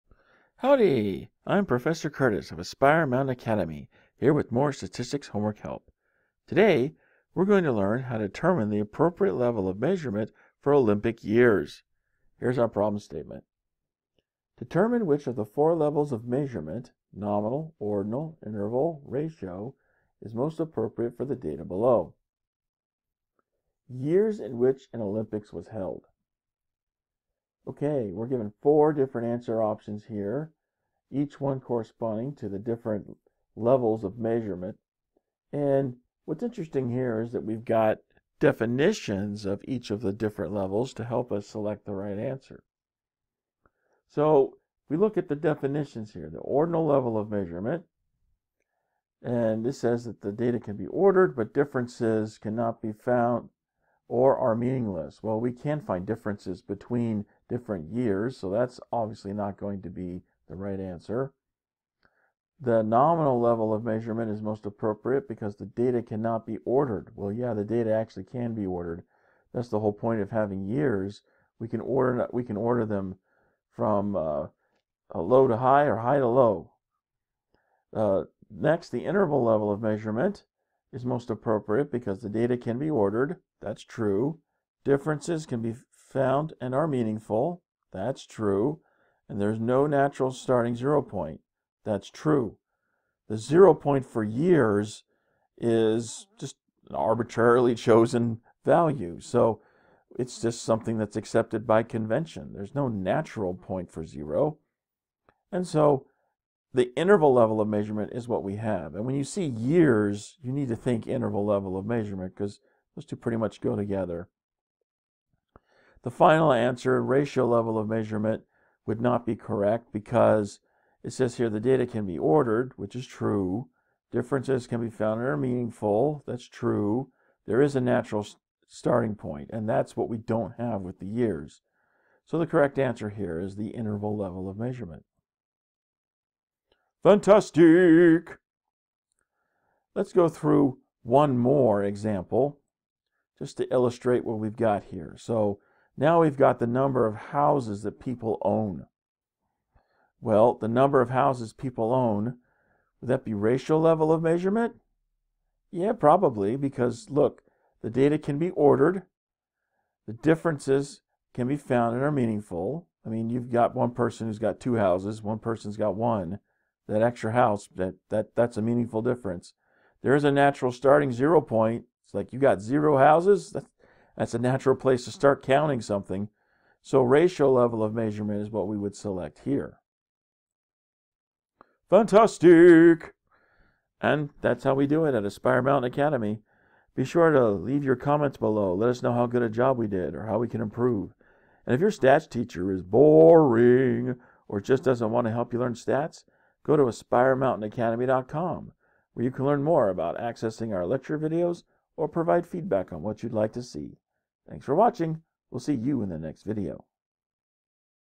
[1.2.26] Determining the appropriate level of measurement for Olympic years
In this video, Professor Curtis demonstrates how to determine the appropriate level of measurement for Olympic years (MyStatLab ID# 1.2.26).